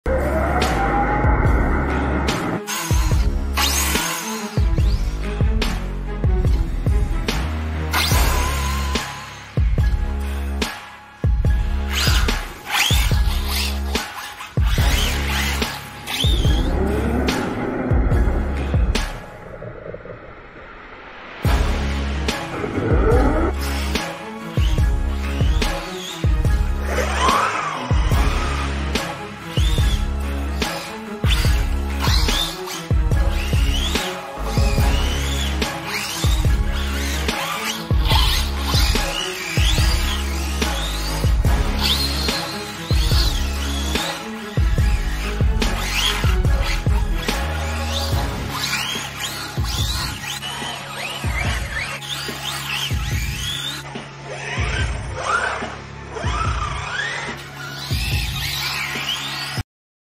Best ARRMA Infraction 😃 Spin + Drift Test !!
Superb drift by ARRMA Infraction !!
Arrma Infraction 🚨drift at parking lot + spinning around and around !!
Fast speeding ARRMA Infraction 👍
Drifting pro !!